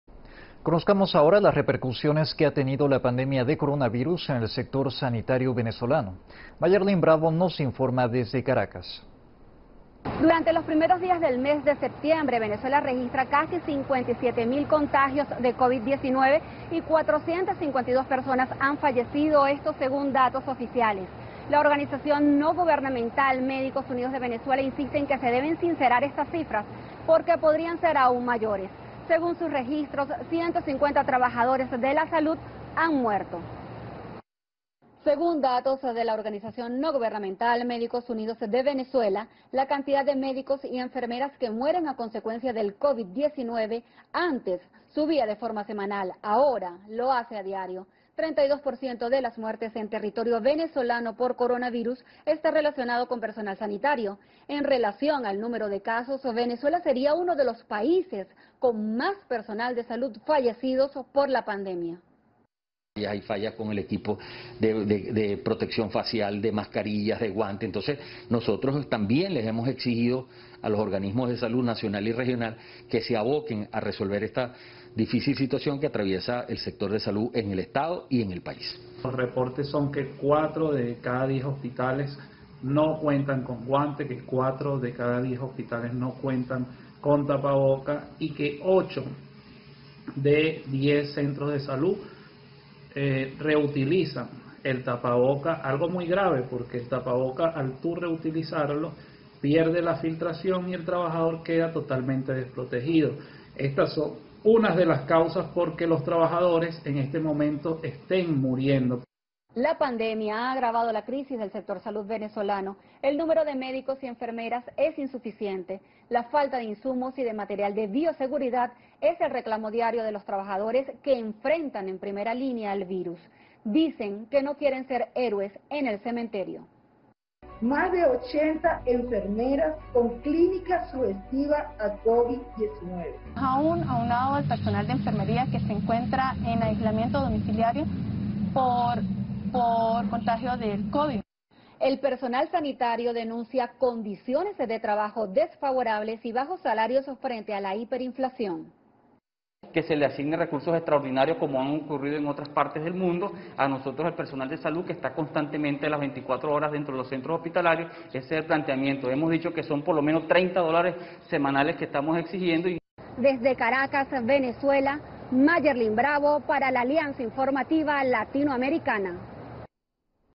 El coronavirus afecta al sector sanitario venezolano
Durante los primeros días de septiembre Venezuela registra: casi 57 mil casos de COVID-19 y cuatrocientas cincuenta y dos personas fallecidas, según datos oficiales... La organización no gubernamental médicos unidos de Venezuela insiste en que esa cifra debe sincerarse, porque sería mayor. Y pide protección para los profesionales de la salud... De acuerdo a sus registros ciento cincuenta trabajadores sanitarios han muerto.